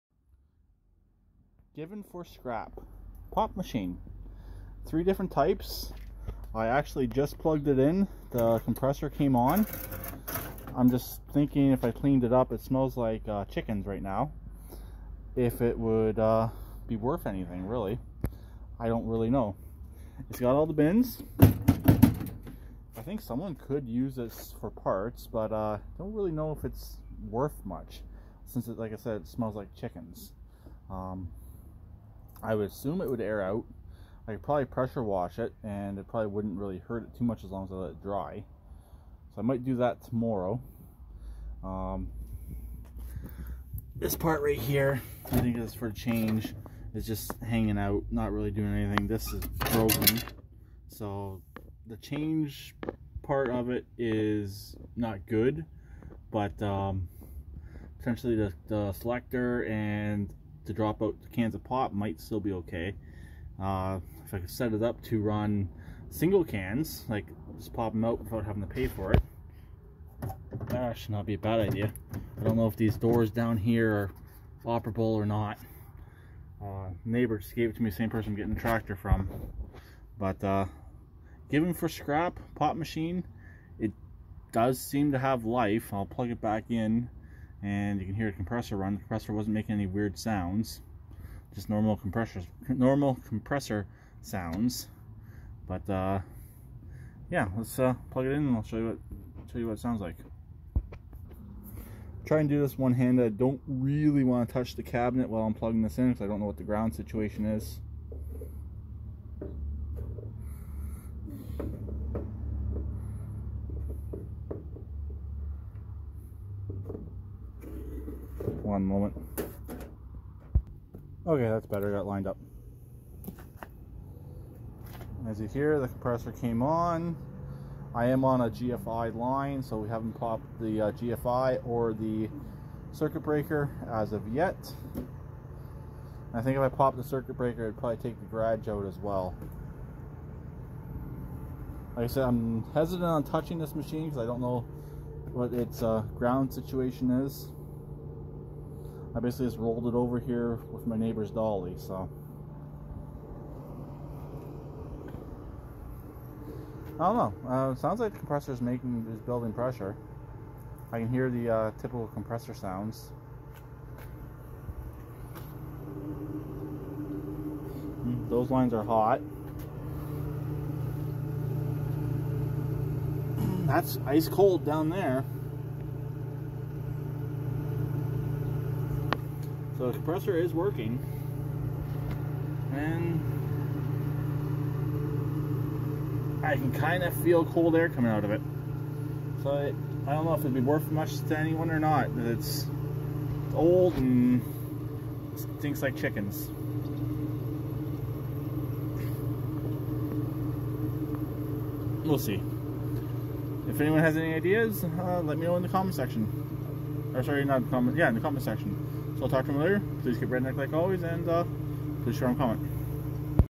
Scrap or Not Pop Machine
Old but not Vintage
This machine was given to me by my neighbor for scrap, but I'm unsure if I want to or not it does appear to power up and get cold.